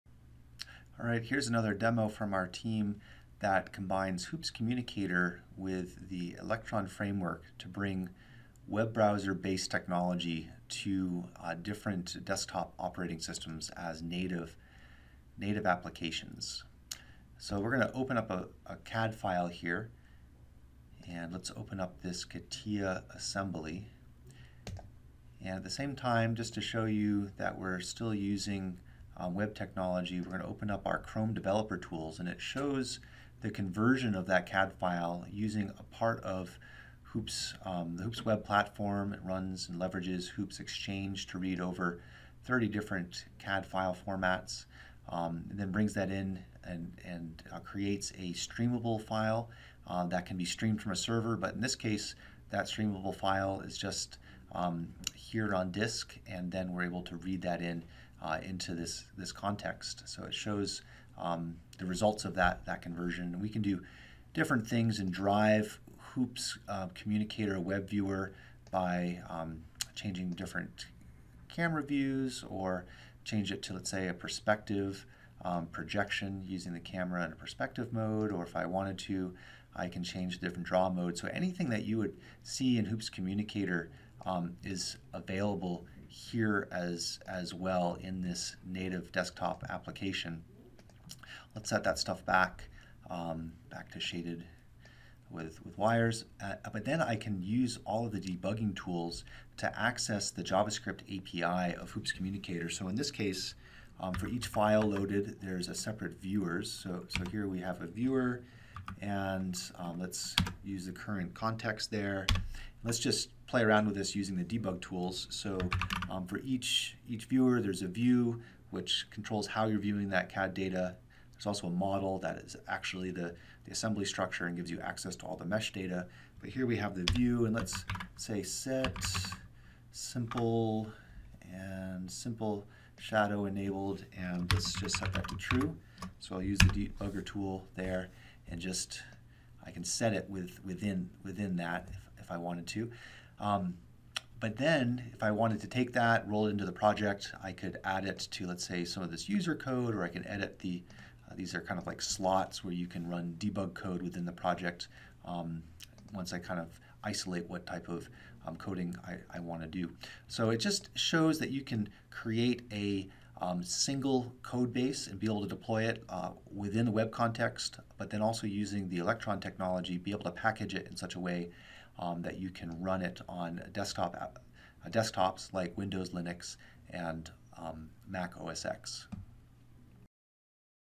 Electron CAD Viewer
Demo combining HOOPS Web Platform with Electron. Creates a native desktop application that can load and interrogate CAD files.